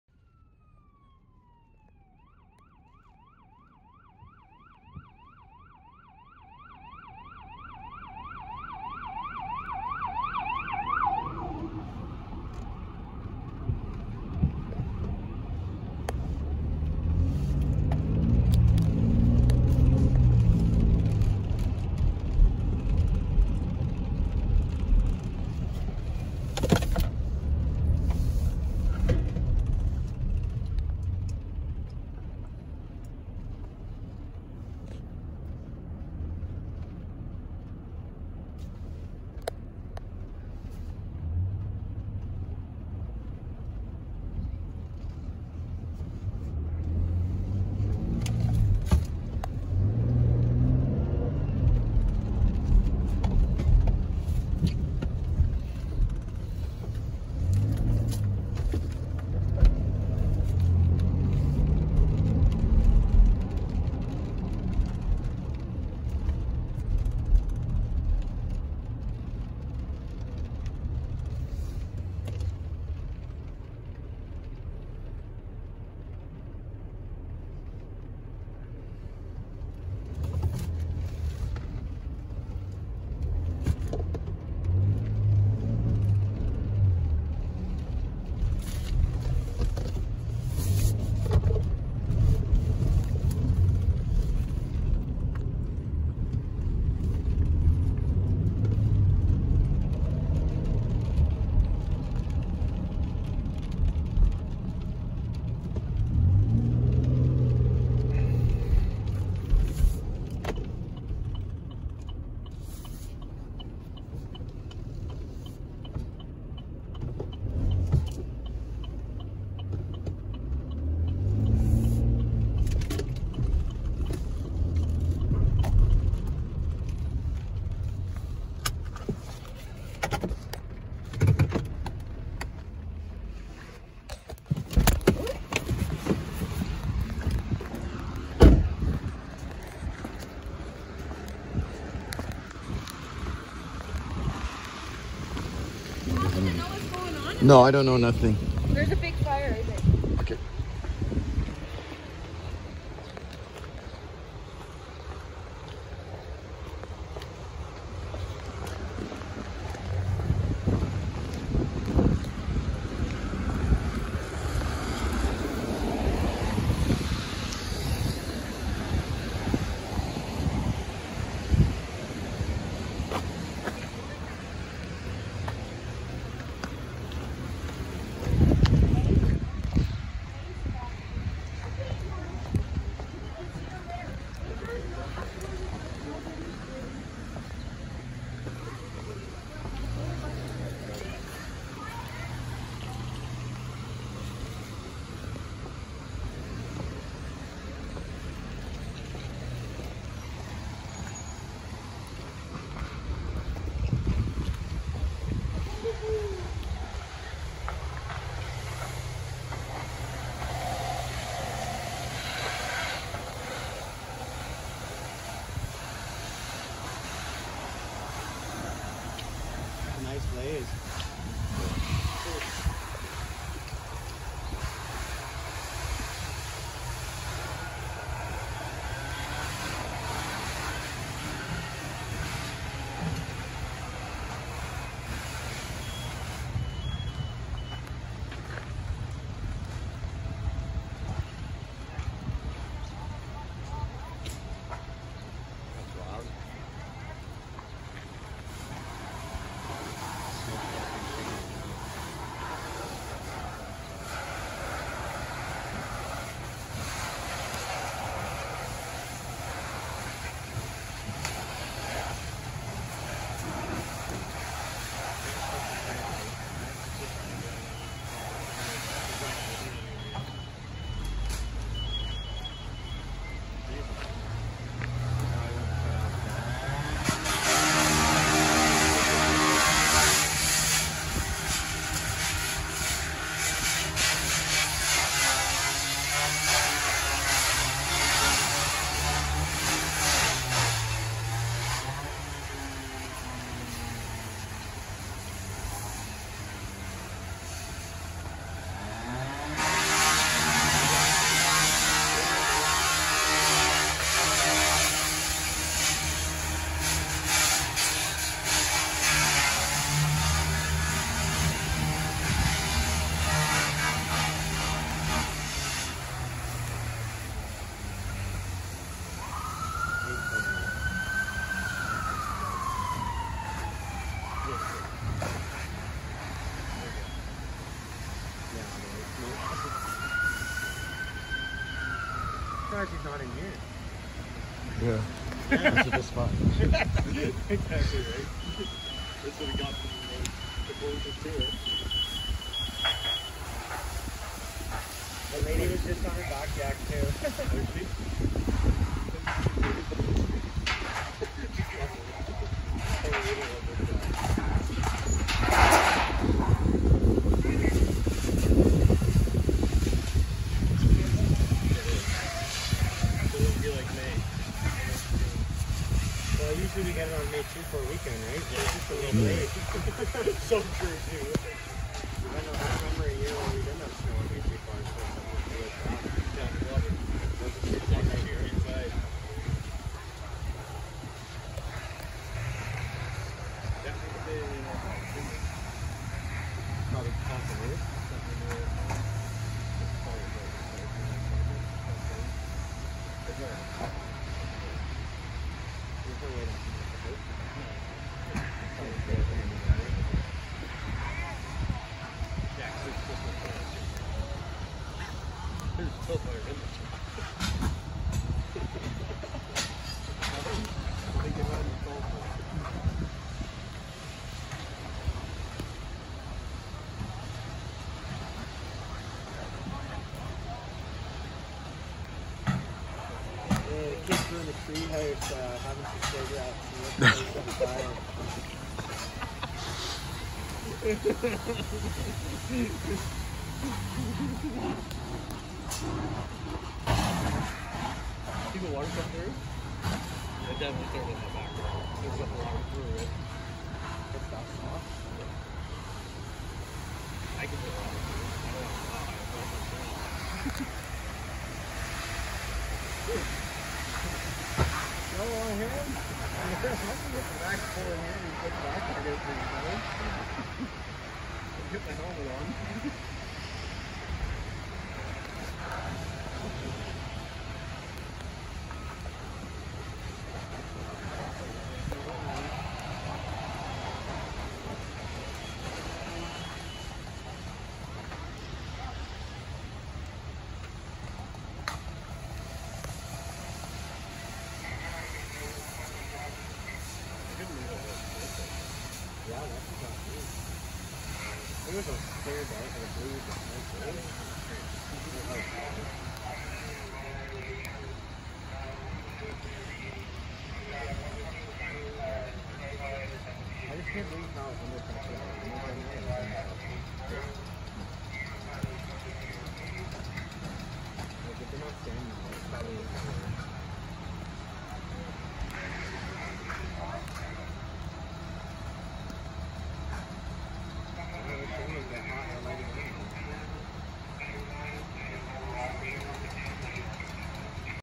Fire in Bowmanville. Liberty and Victoria St. May 28, 2021.
A shed with a wood burning chimney was on fire.   Could not access streets so i found a back parking lot of an apartment building to get this footage.  Fire was on Victoria St,  by Liberty St.